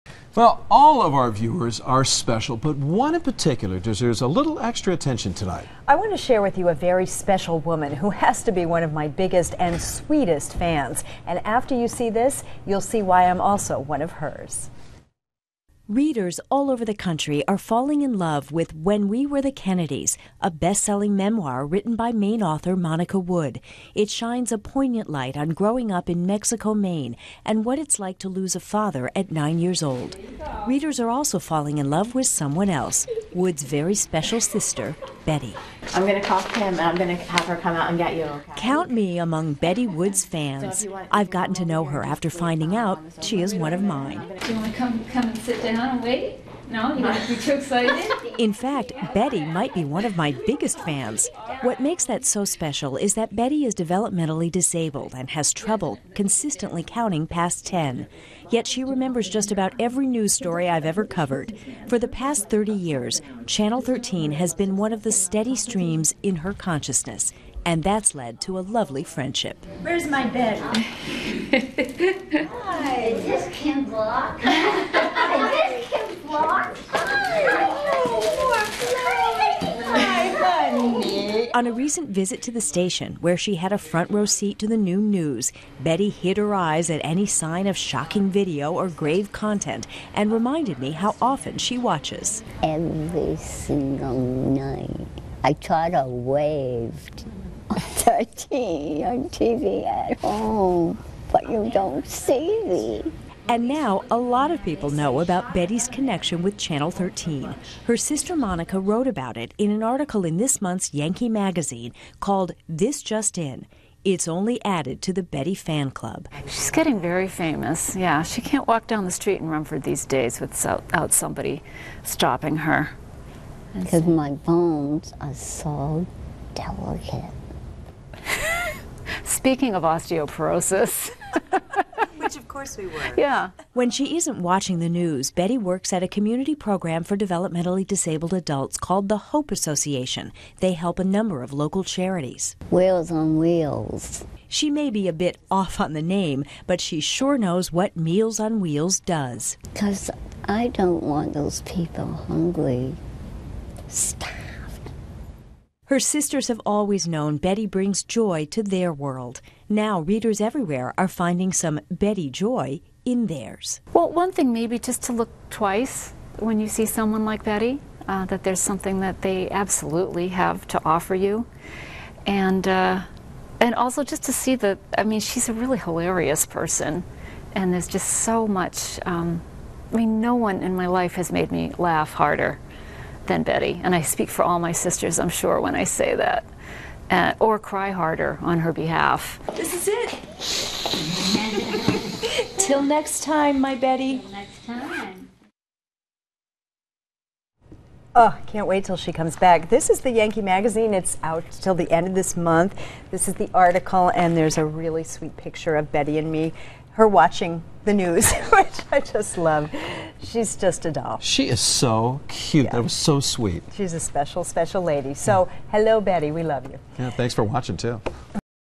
SPECIAL REPORT: Kim's biggest fan, as seen in Yankee Magazine
RUMFORD (WGME) -- All of our viewers are special. But one, in particular, deserves a little extra attention. Kim Block shares with you a very special woman, who has to be one of her biggest fans.